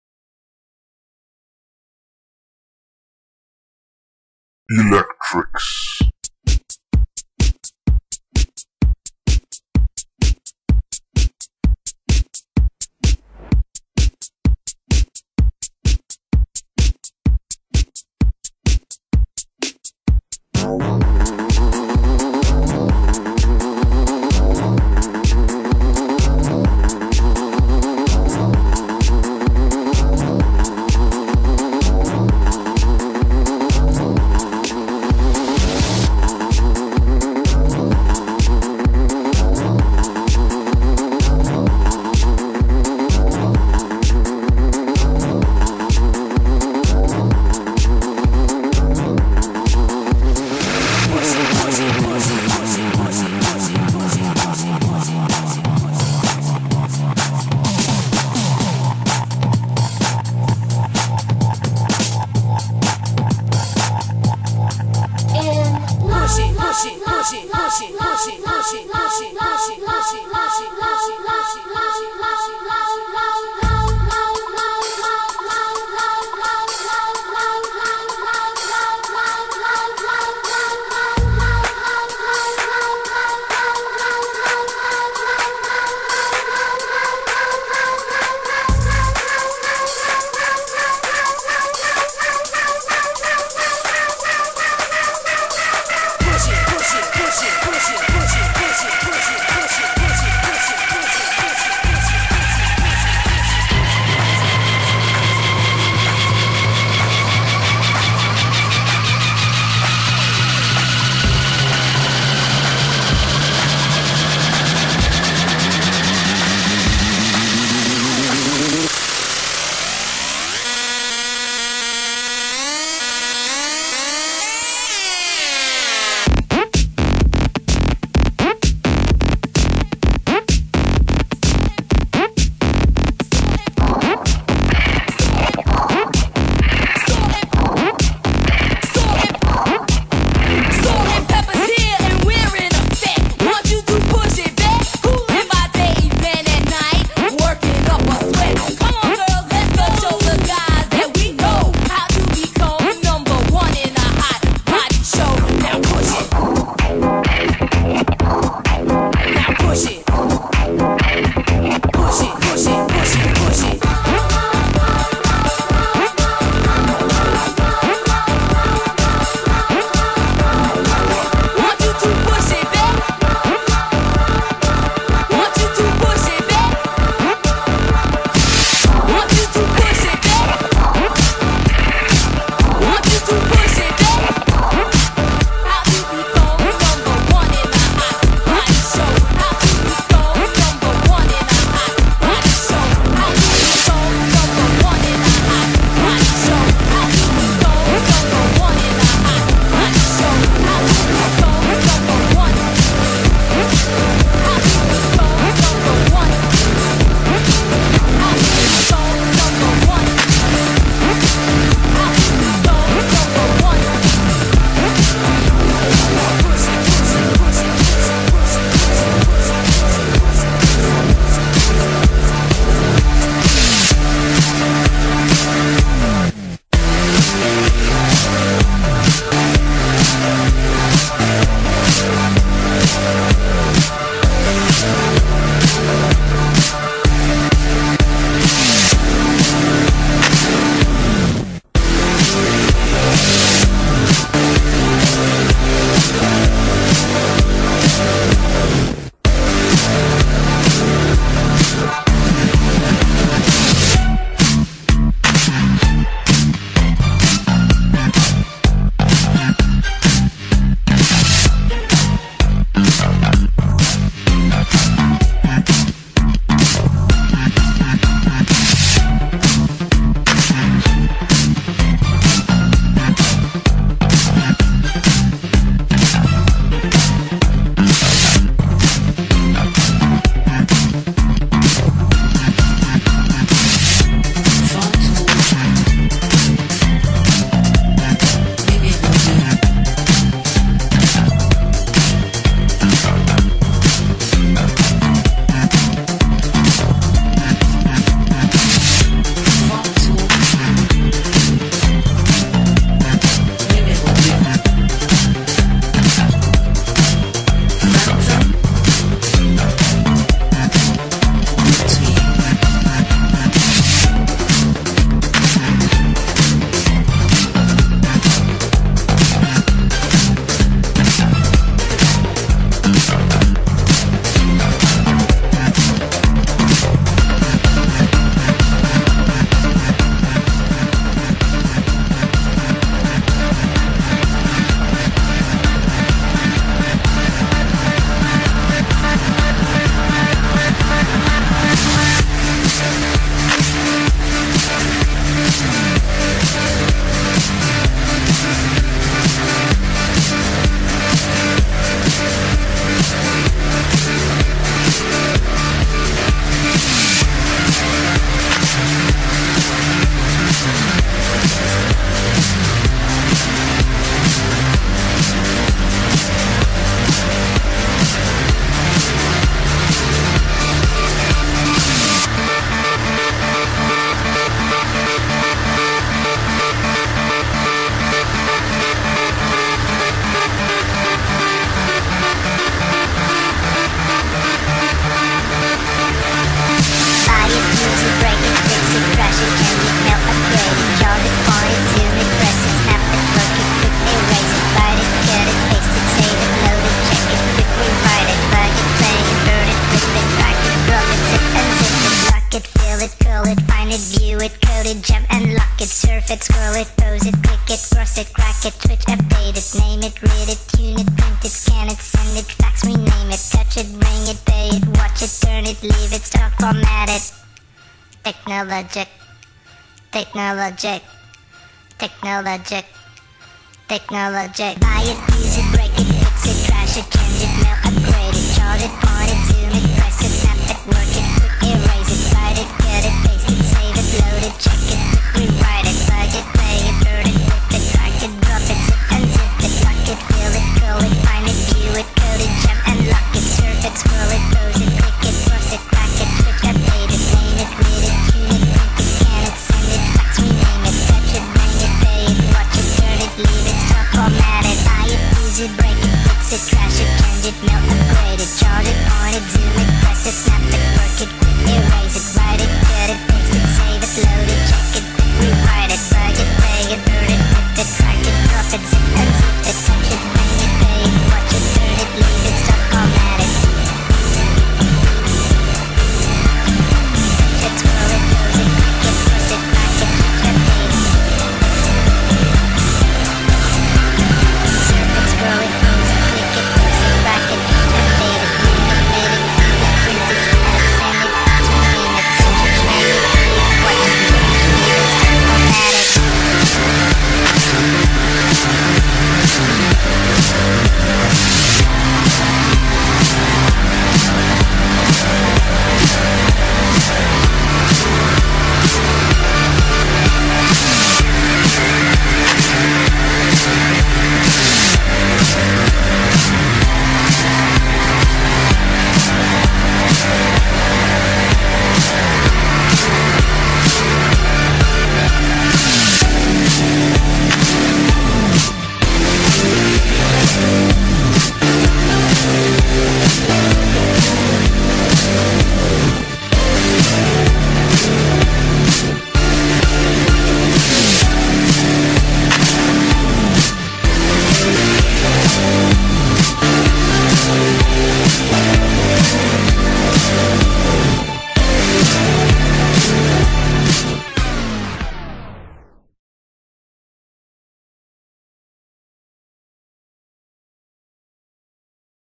100 dagen PHTI 2009
6 maart 2009: 100 dagen

Een beknopte voorstelling van onze eindjaren, en hun verschillende acts.

Muziek door de jongens van 7PR en hun DJ-set-groep Elec-trix.

Photos by Petitlion